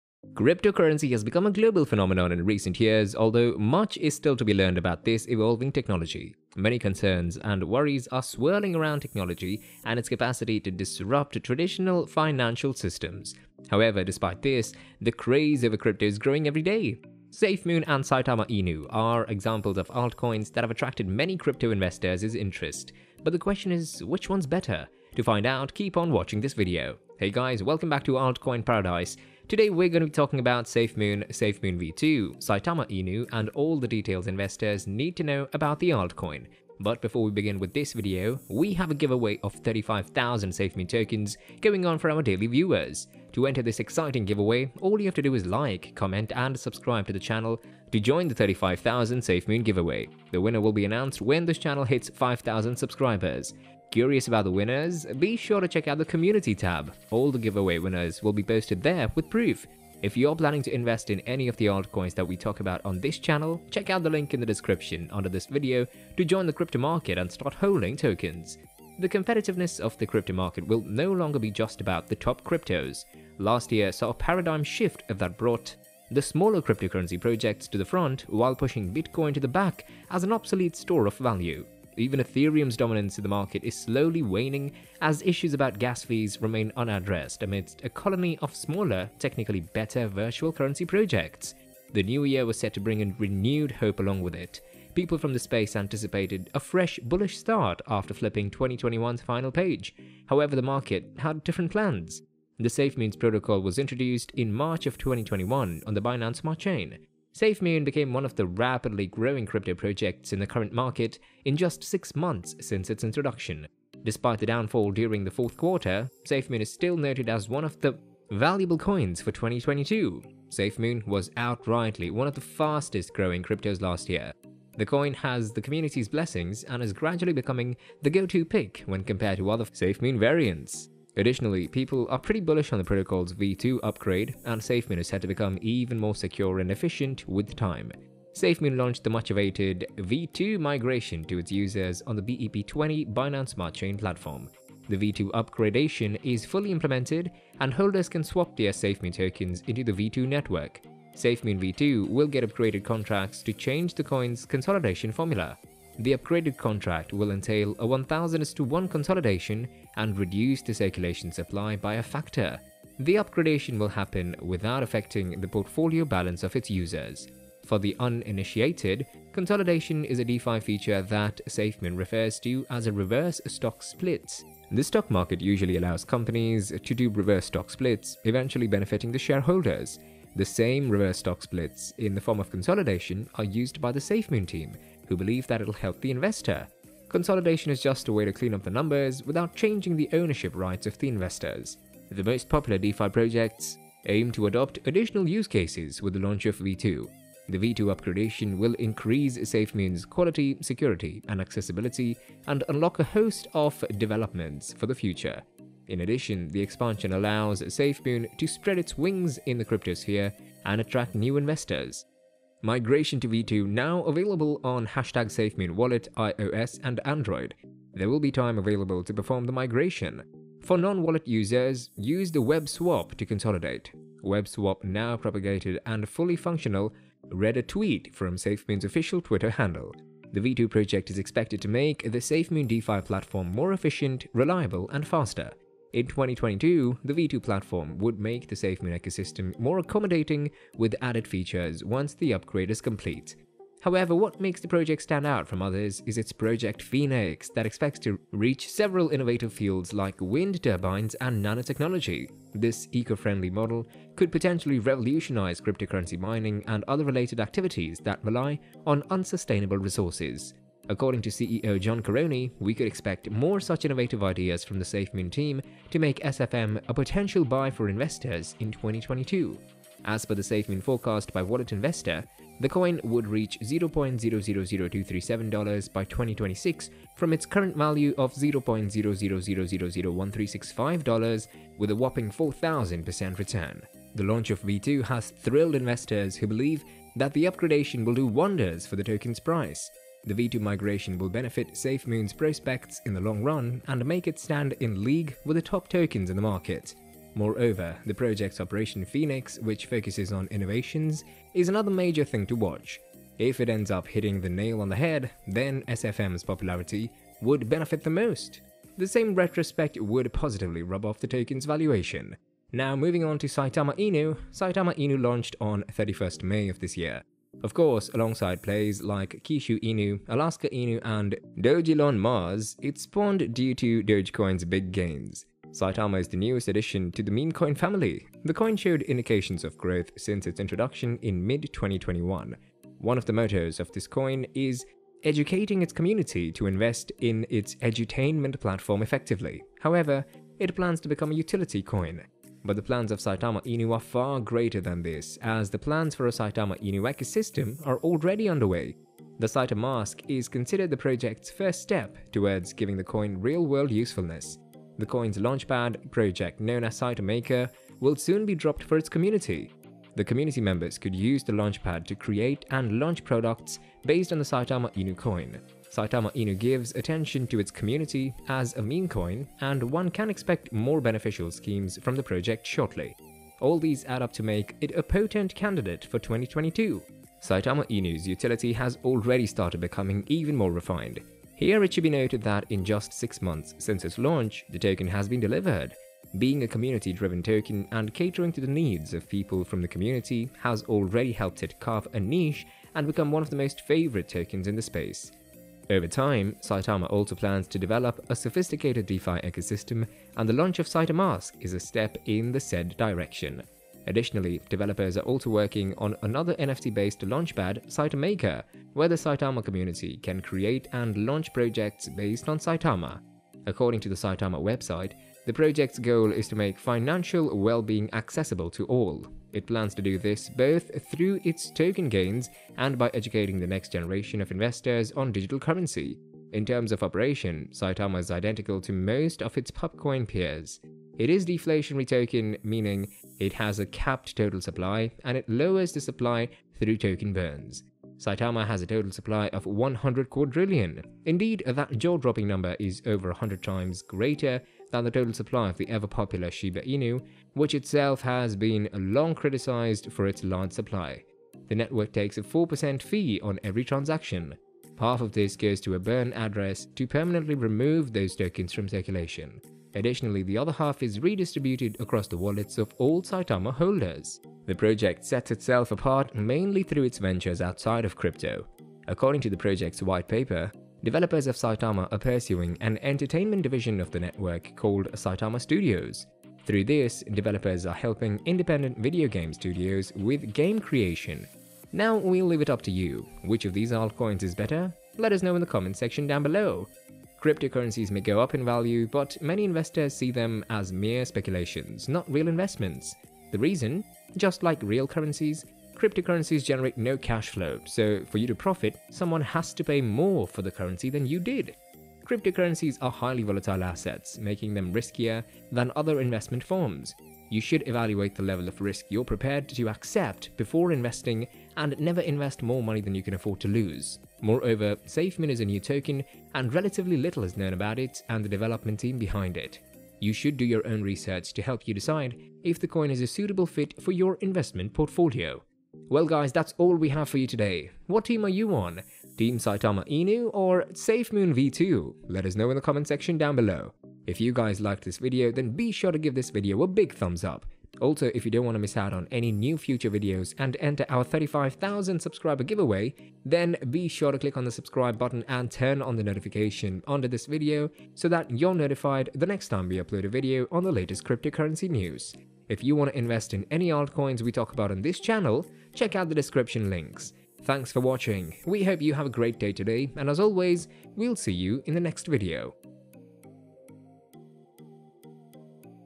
SAITAMA INU VS SAFEMOON!! WHICH ONE WILL MAKE YOU A MILLIONAIRE?!! SAFEMOON NEWS TODAY!!
Today in this video we're gonna talk about everything Safemoon, Safemoon 2021, Safemoon News today and Safemoon price. The crypto space is always surprising us, so try to keep up with the cryptocurrency news.

About Altcoin Paradise
🎥 Videos about Altcoins and Crypto trends
🎨 Written, voiced and produced by Altcoin Paradise